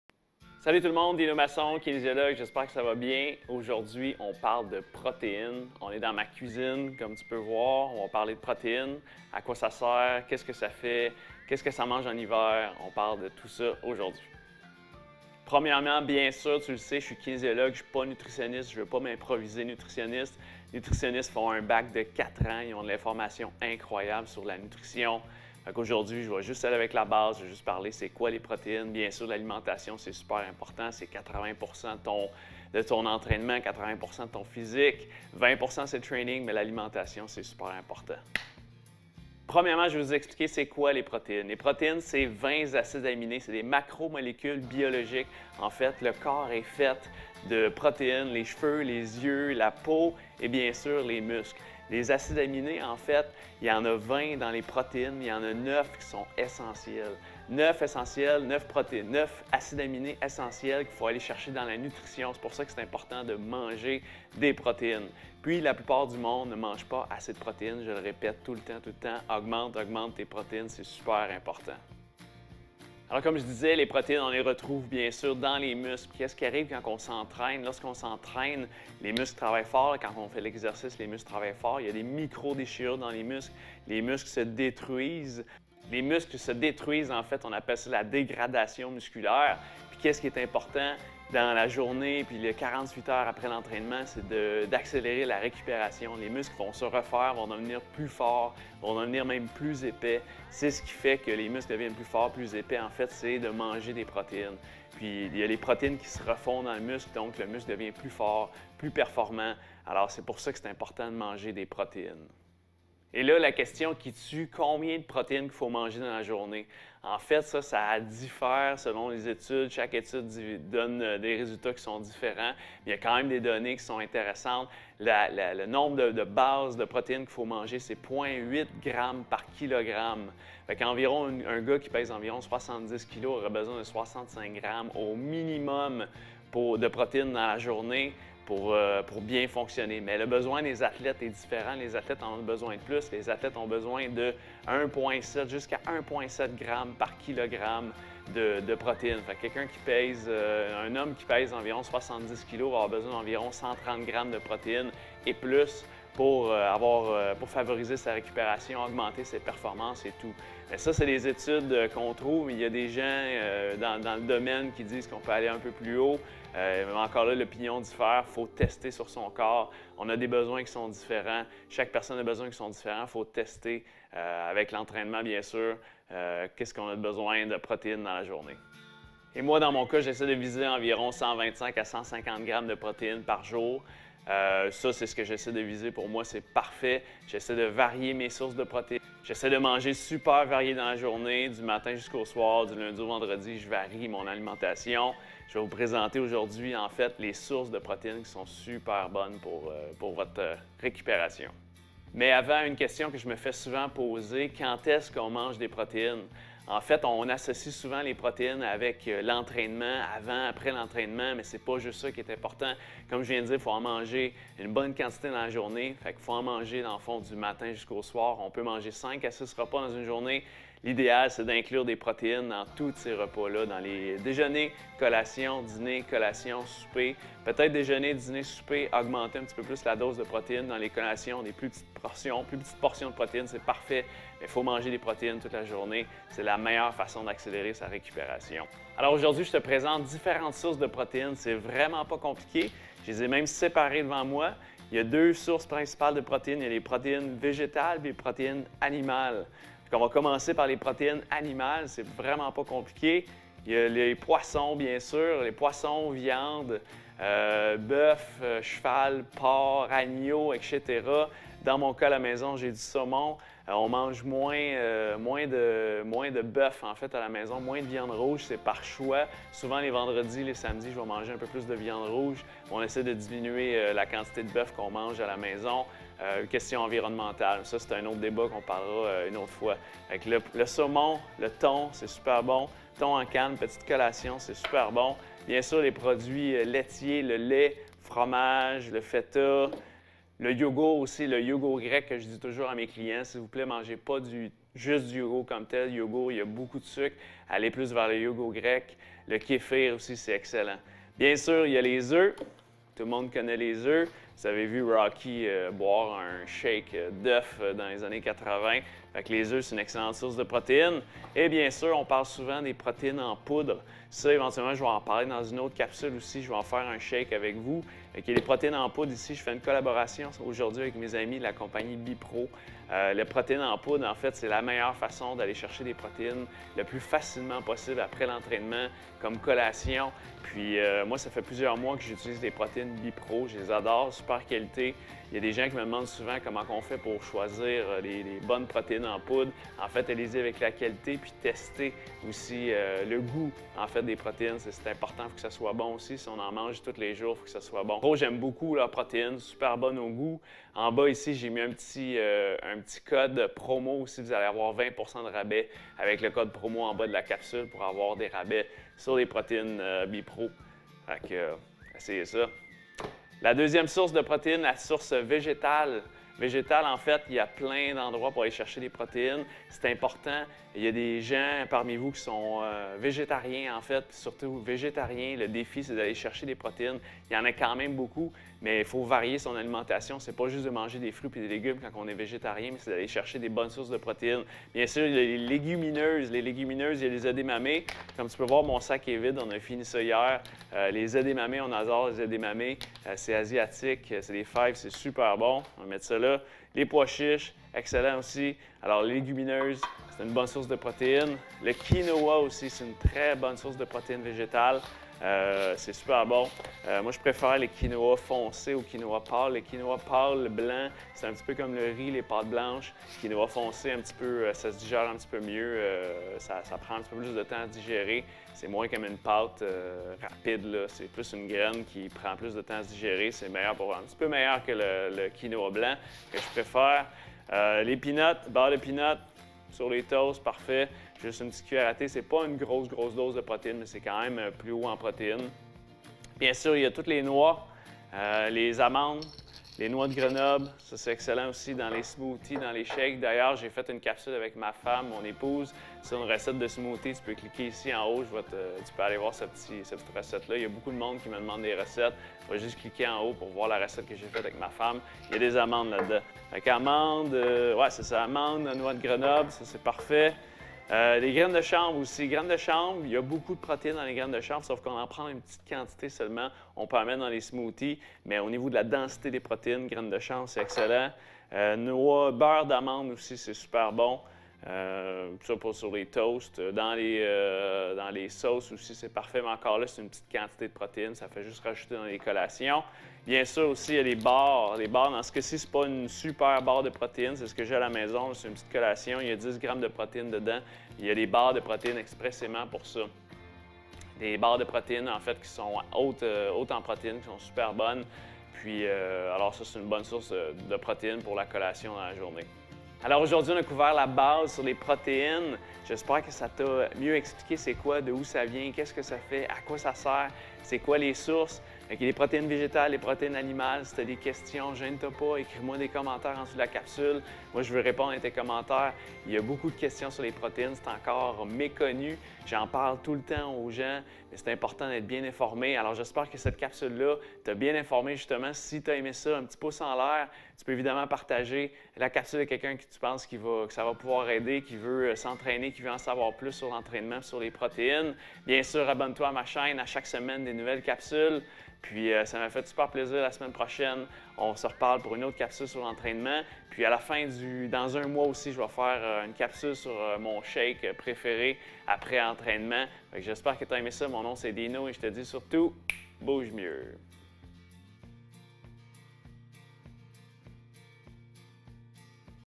Les Meilleures Sources de Protéines pour les Muscles
Aujourd'hui, parlons nutrition! Plus précisément des protéines!

De quoi sont faites les protéines? Pourquoi est-ce que c'est important de manger des protéines? Quelle est la bonne quantité par jour?

J'ai fait cette vidéo en collaboration avec mes amis des nouvelles protéines de lactosérum biPro que j'adore depuis plusieurs mois!

Entre le code biproc20 et obtiens 20% de rabais sur ton achat sur Amazon!

Je réponds à toutes ces questions de base sur les protéines. Je suis kinésiologue, et non pas nutritionniste, donc je vous parle seulement des éléments de base. Chacun a ses besoins différents et c'est avec le temps et les efforts que l'on découvre ce dont on a besoin.

Regardez la vidéo pour en savoir plus!

Bouge mieux! Bon entraînement! À jeudi prochain pour notre programme de remise en forme 101!

Filmé avec ma GoPro Hero 5